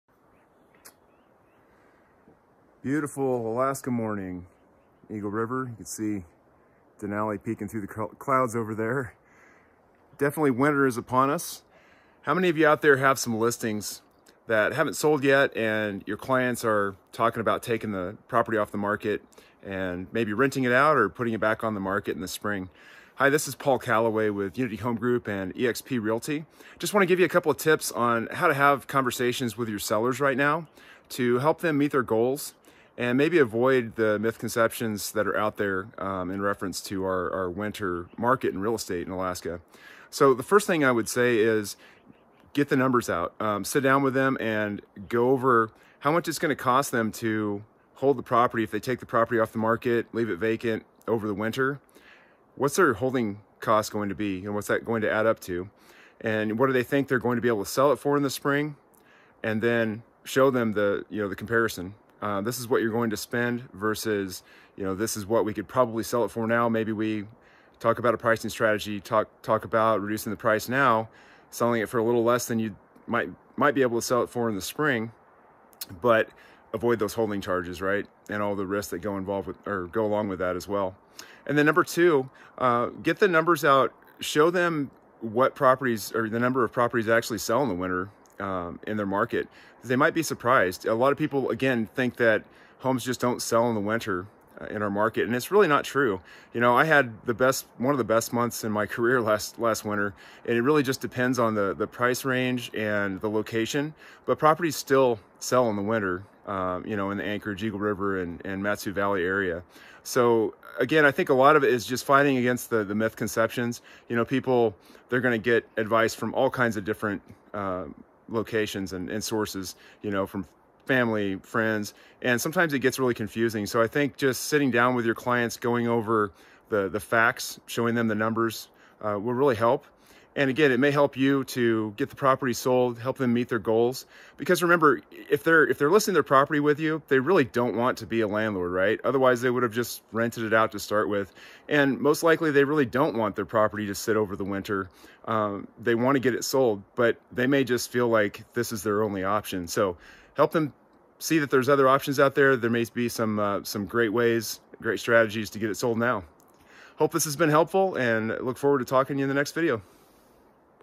Need to get a home sold before the sellers take it off the market or rent it out for the winter?
Need to get a home sold before the sellers take it off the market or rent it out for the winter? Find out how!￼

🔑 Meet with your sellers in person and show them a cost comparison detailing their holding charges over the winter versus what they could sell the home for now and what they need to do to make it sell.

📈 Show the seller statistical data from the multiple listing service which explains how many homes typically sell over the winter in their price range and location. They may be surprised by the facts versus the myths that they’ve heard about properties not selling in the winter.

☝ Remember, if the sellers really wanted to be landlords or have the property stay in their possession until spring they most likely would not have listed with you. Help them sort through all of the misconceptions and find a way to sell it now.